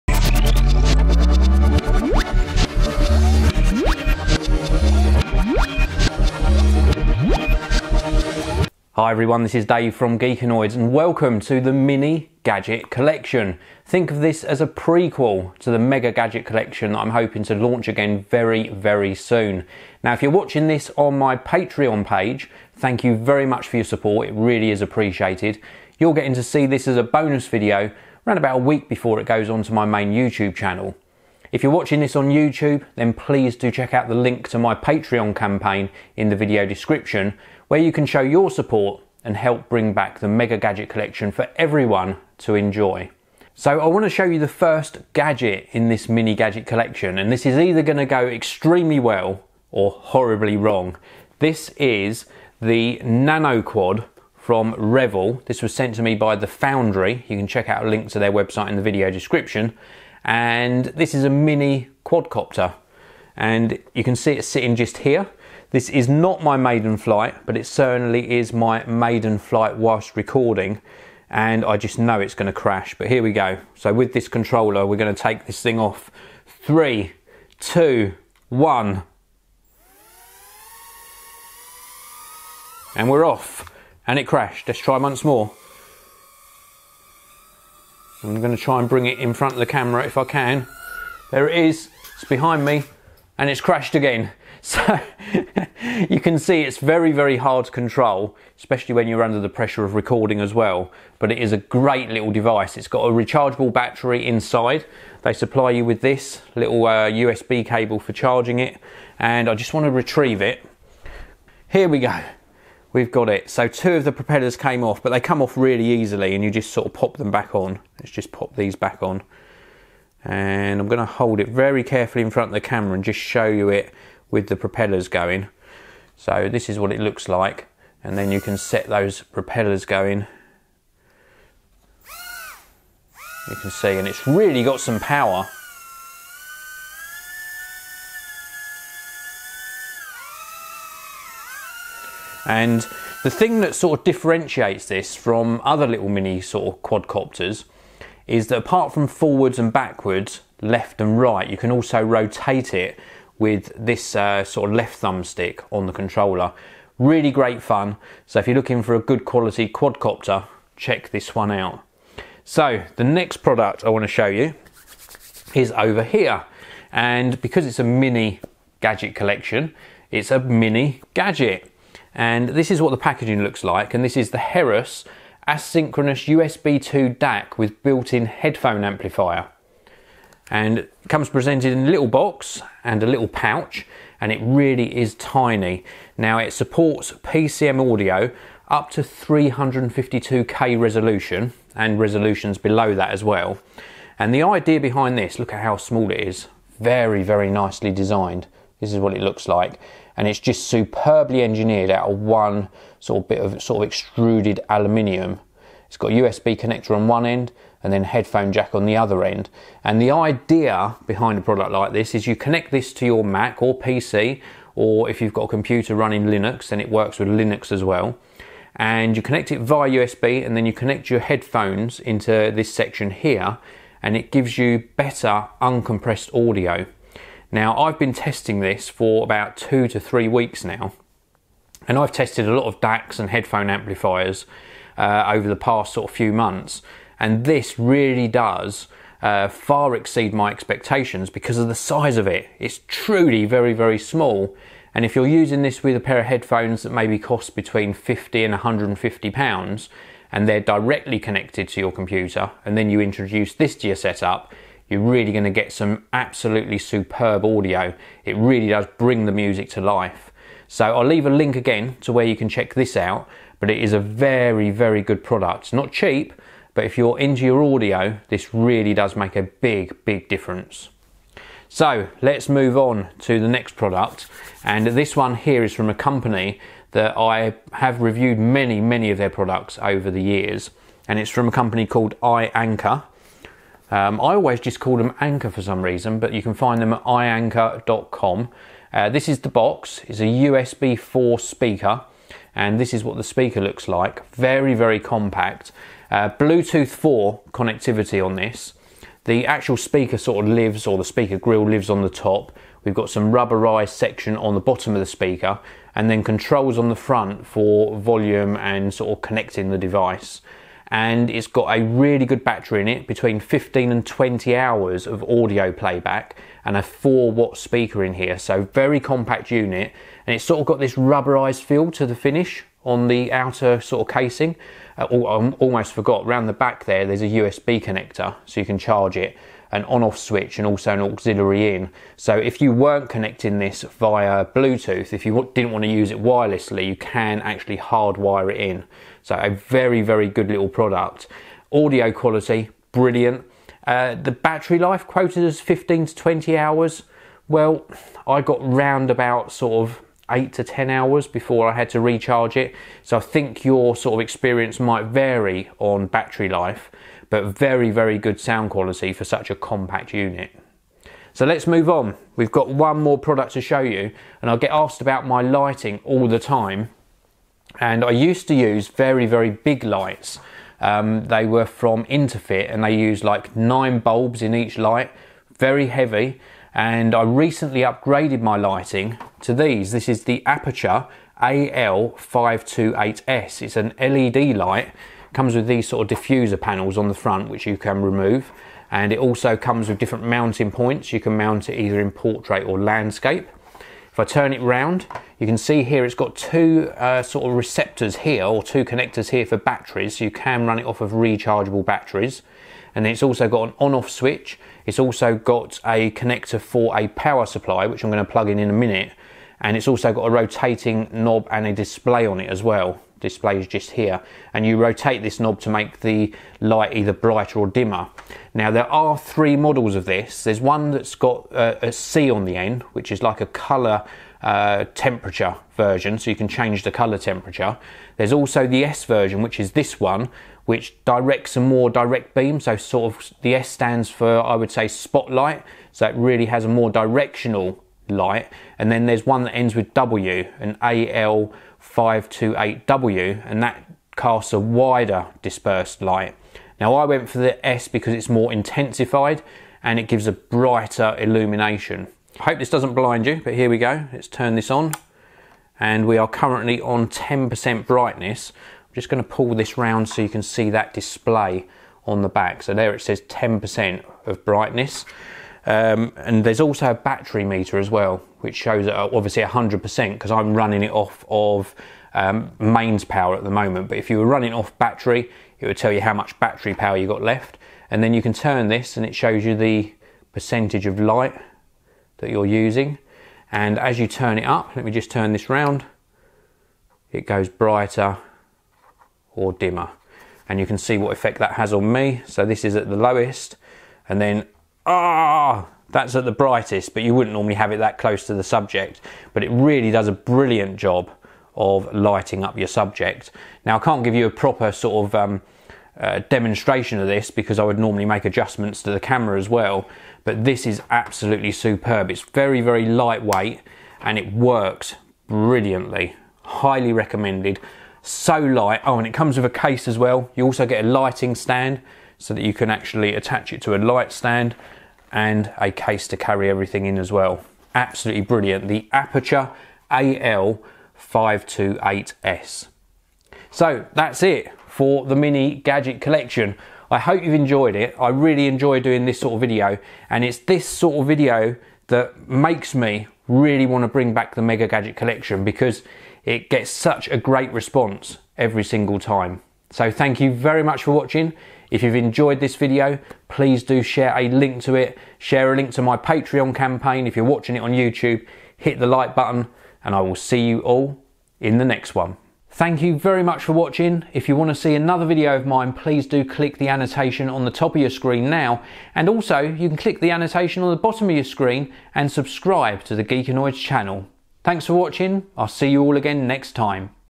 Mini Gadget Collection #1
Mini Gadget Collection #1 ... Originally published 15th February 2014 for Patrons. This is a prequel to the relaunch of The Mega Gadget Collection.

Products featured ...
Anker Bluetooth Speaker or USA Only
Aputure AL-528S LED Light UK - USA

Our Patron Supporters ...

Download free Apps on iOS & Android and earn FREE money like me